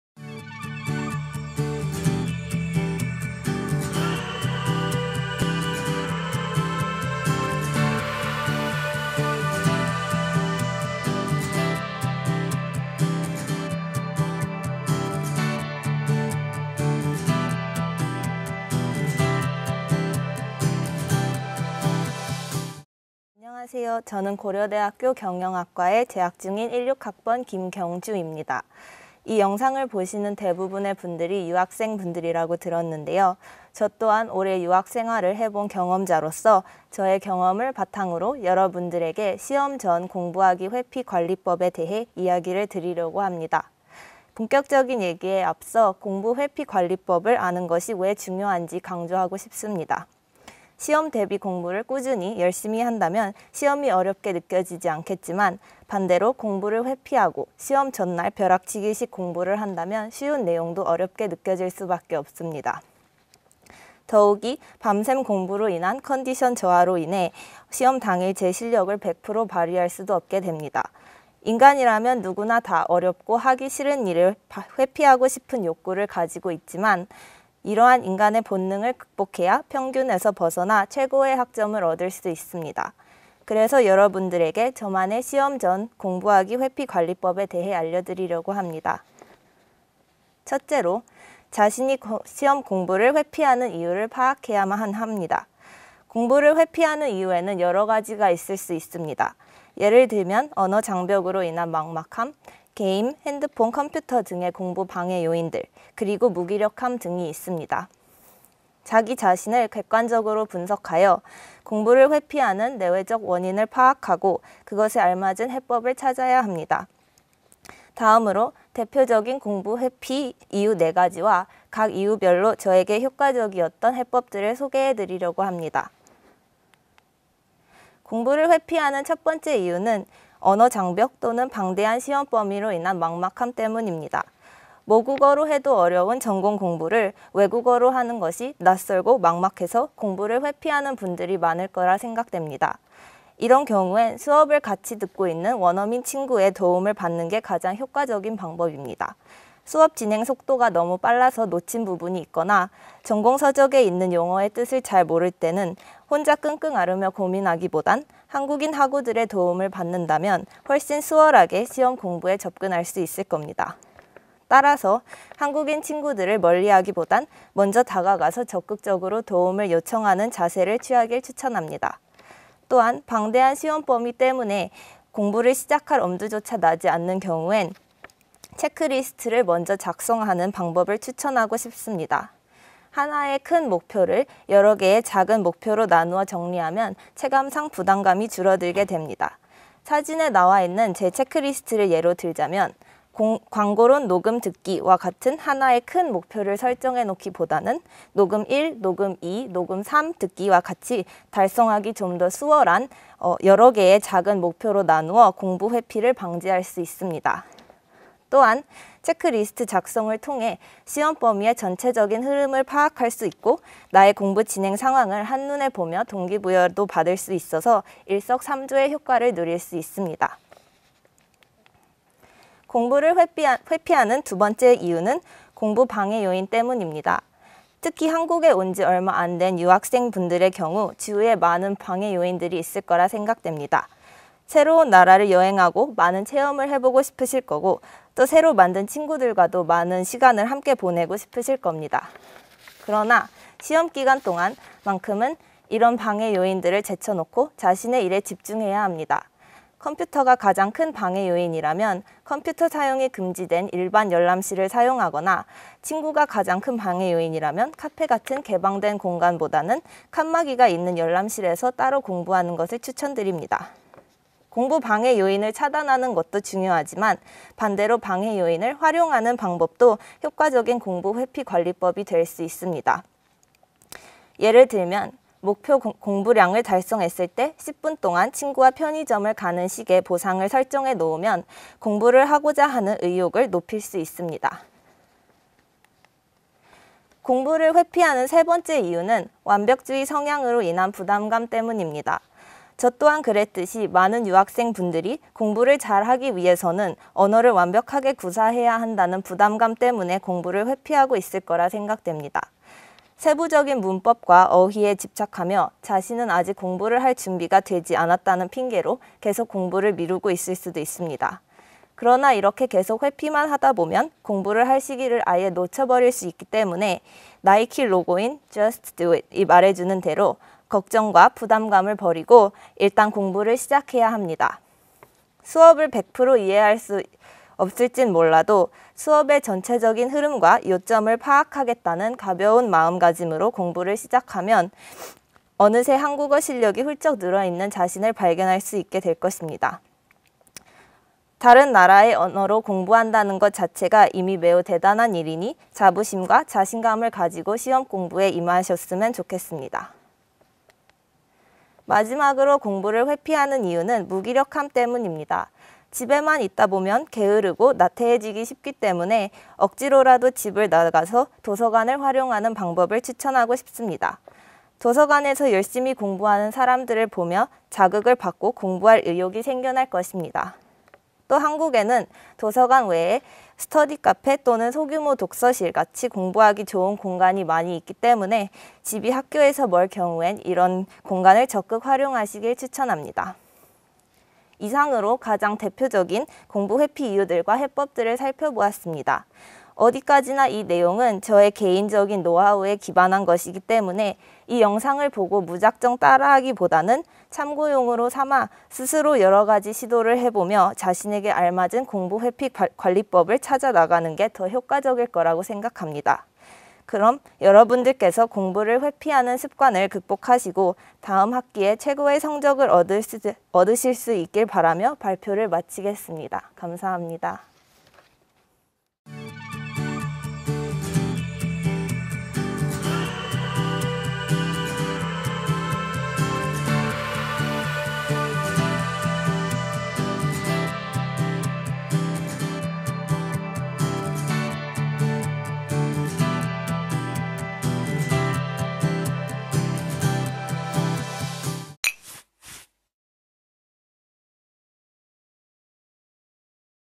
[고려대학교 Korea University] 단과대학 최우등생 학습전략_경영학과 김경주
단과대학 최우등생 학습전략
경영학과  16학번 김경주
-시험 전 공부하기 회피 관리법-

주관 : 고려대학교 대학교육개발원 교수학습지원팀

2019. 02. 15

고려대학교